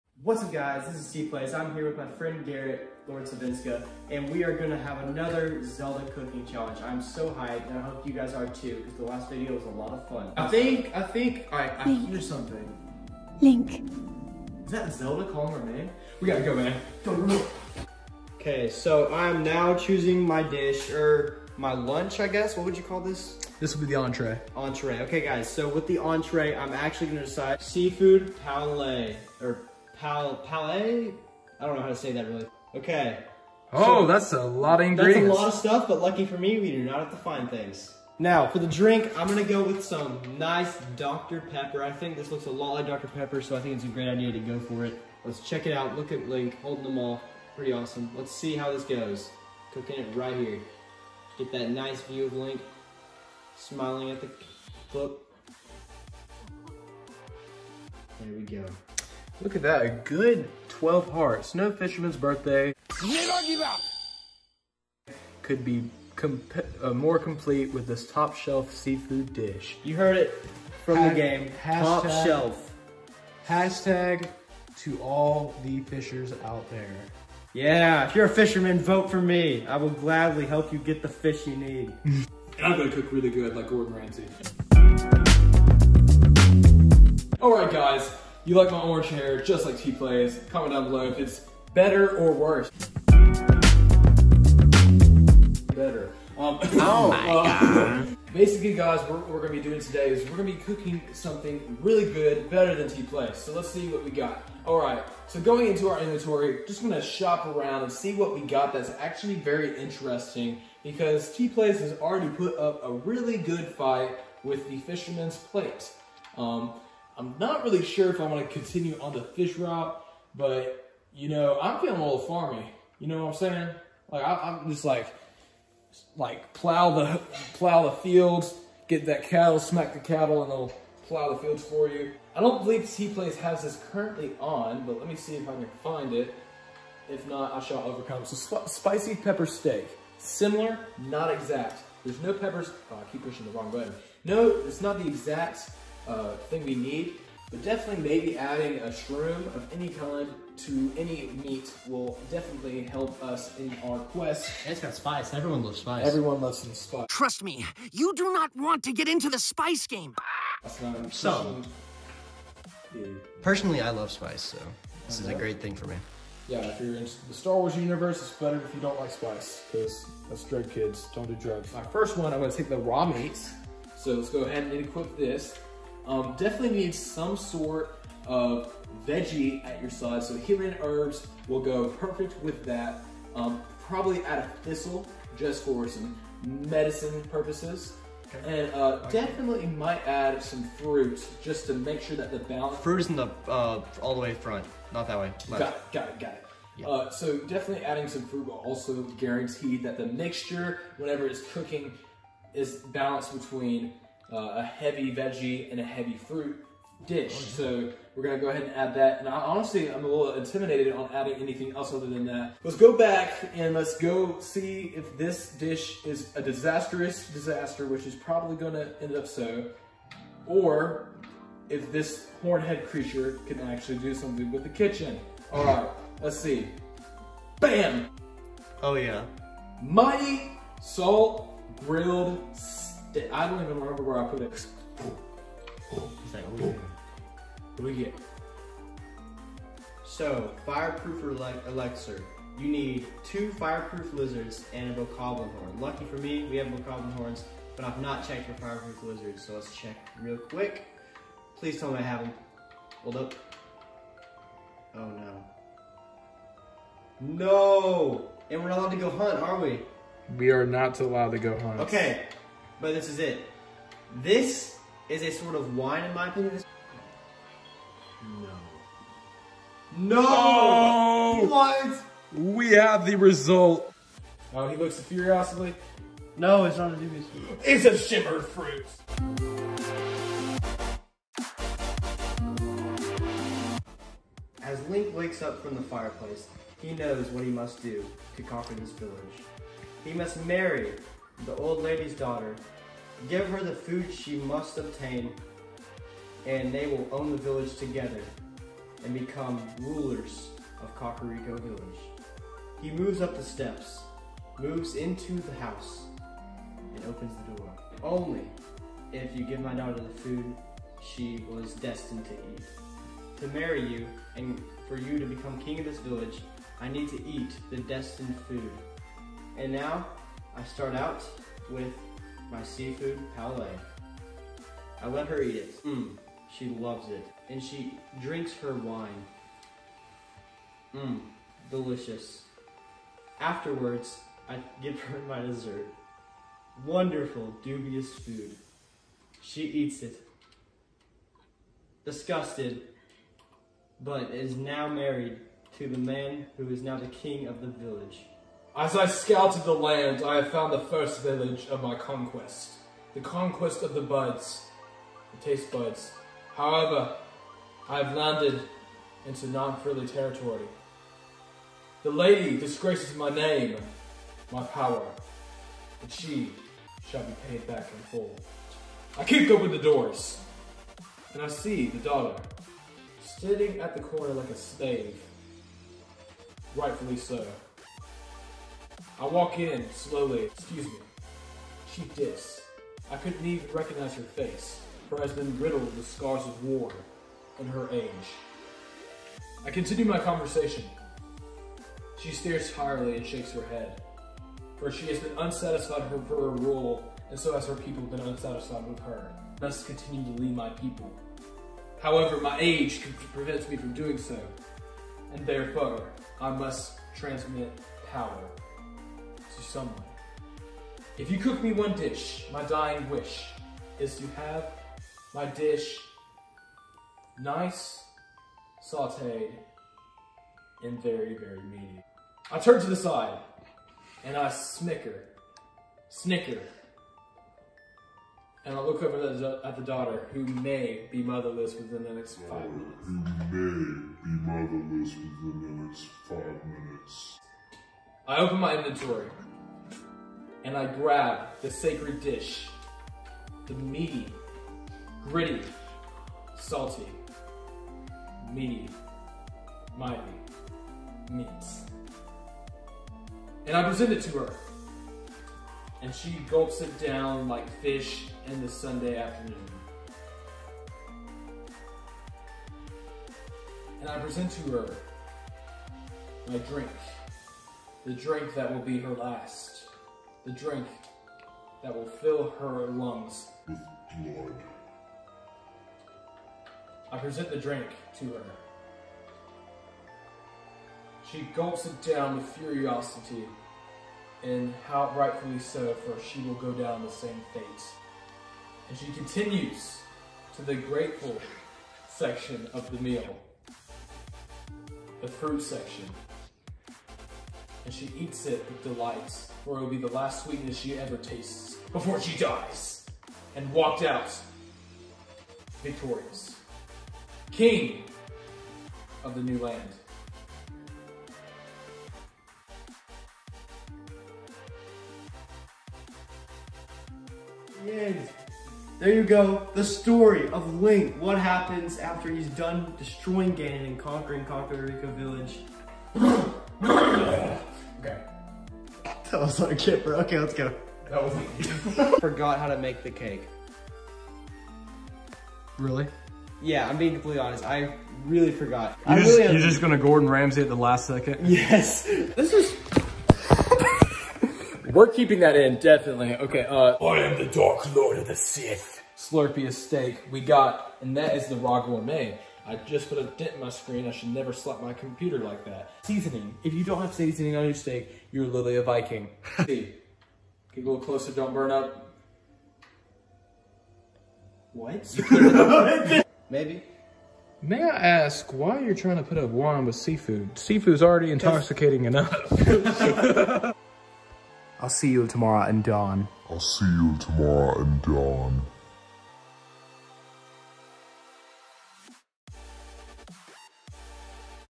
The Zelda cooking competition #2
The second part of our cooking challenge series, comment bellow what we should try next.